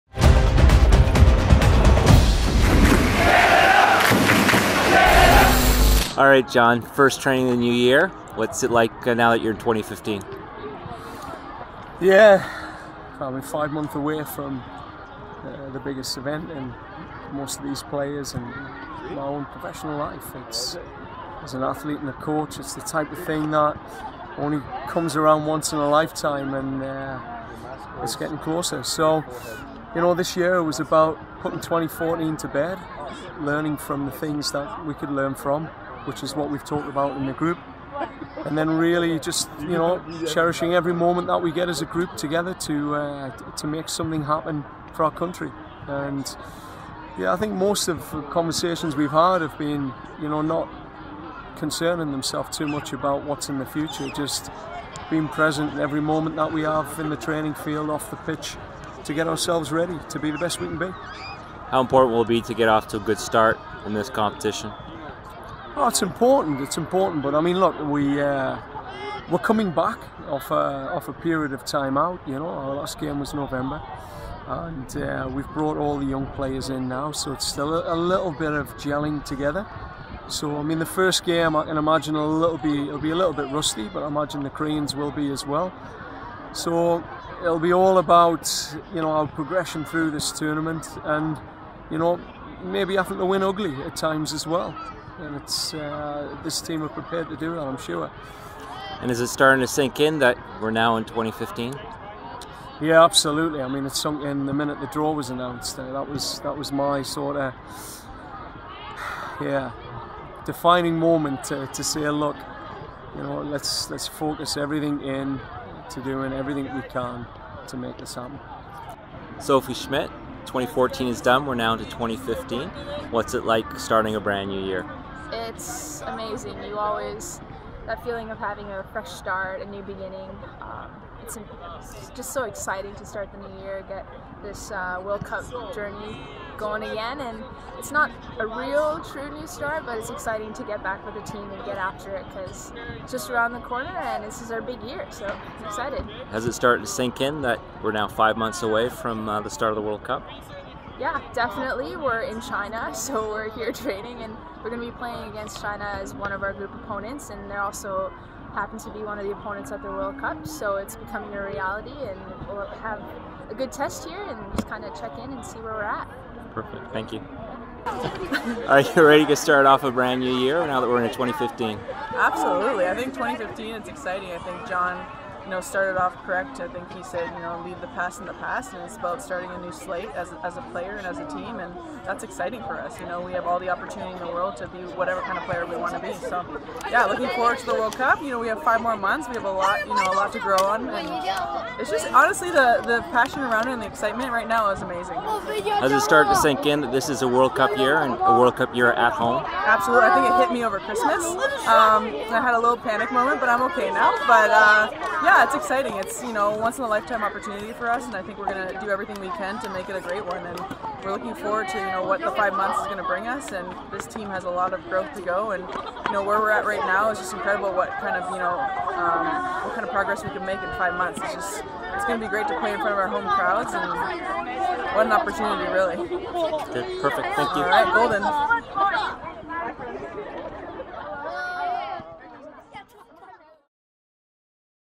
CANWNT : Herdman & team preview an important 2015
Canada's Women's National Team was back at training ahead of China's Four-Nation Cup, reflected on the year that is beginning, and the importance of their preparation for the FIFA Women's World Cup Canada 2015.